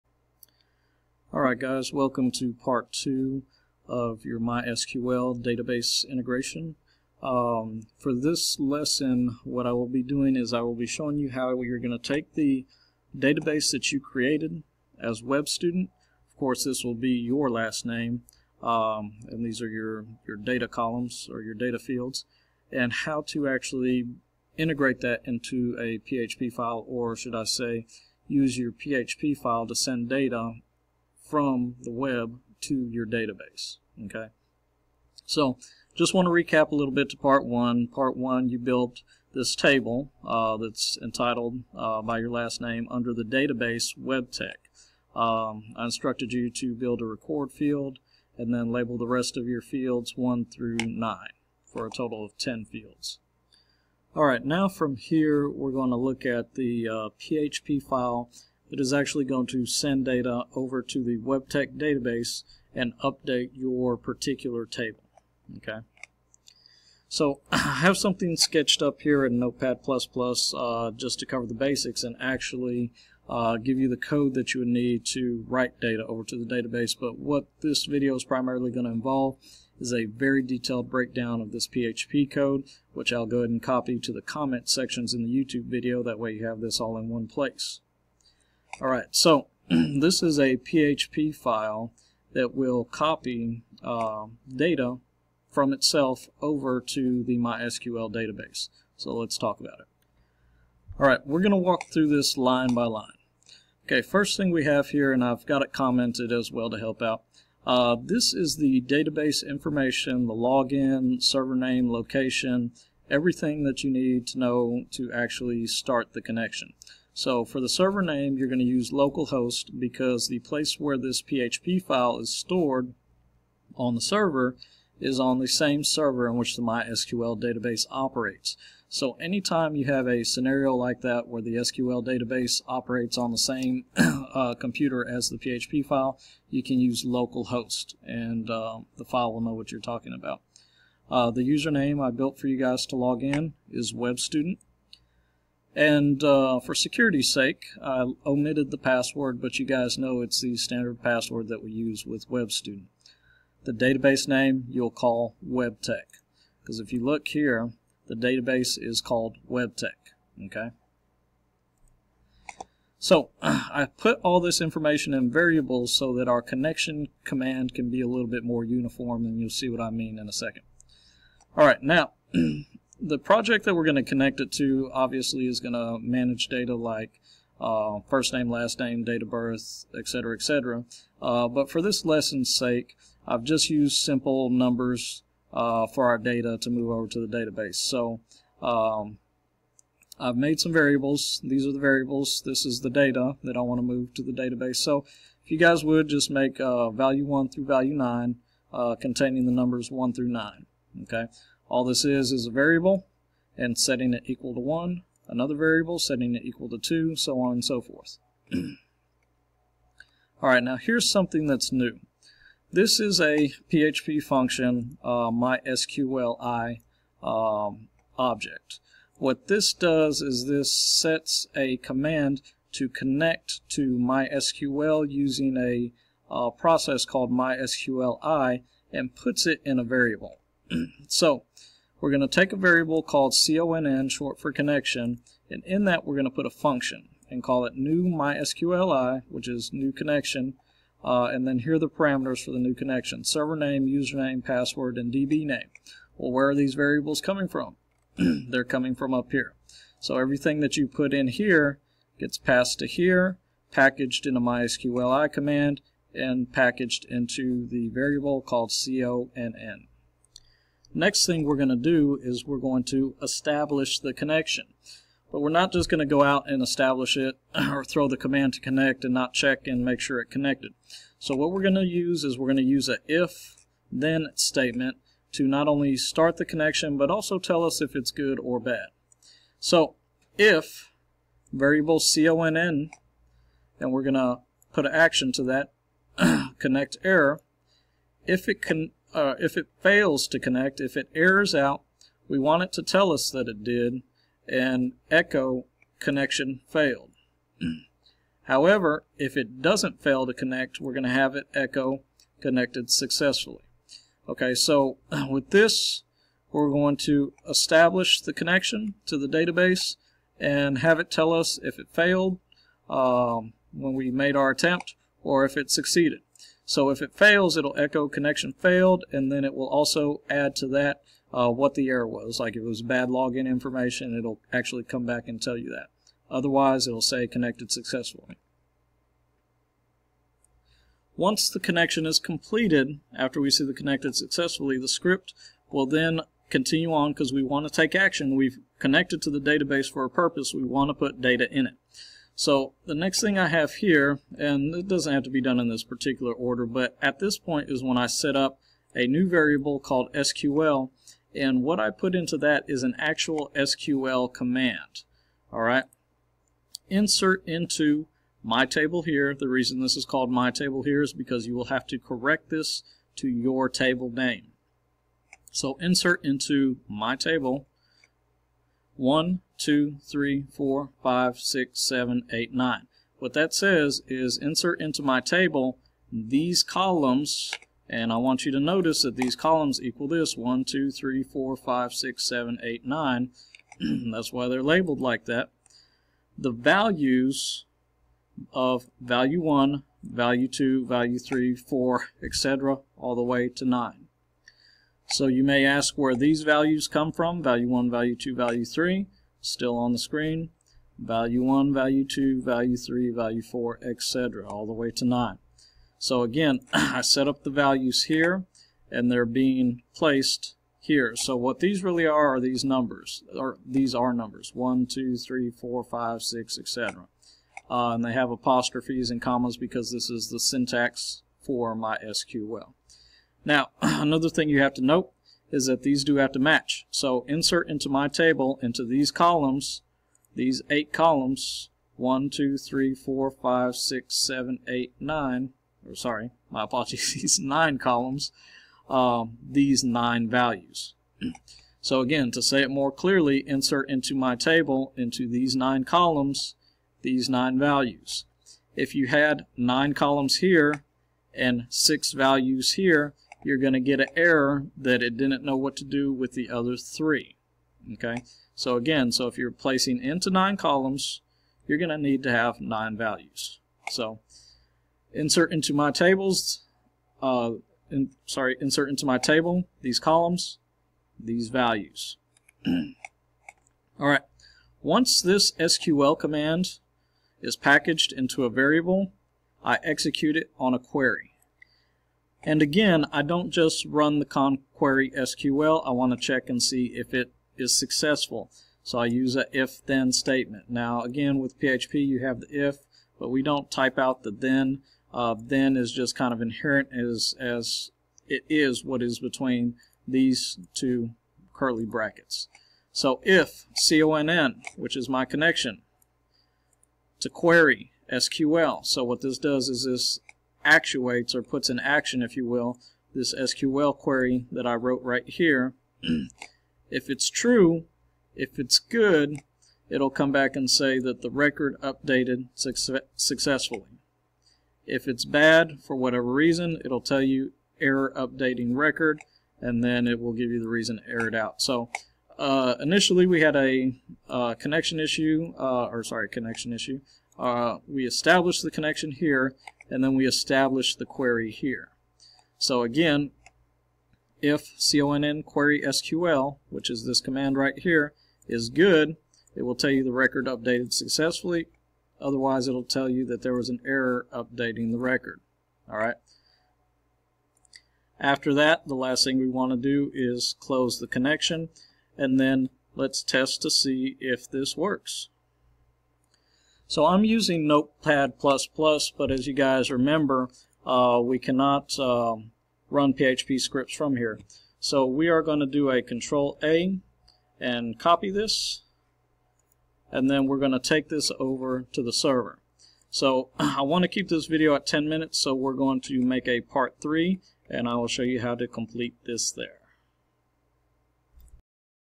Digital Bytes: Understanding the PHP MySQL Connection Part 2
A walkthru the PHP file and how it interacts with the MySQL file.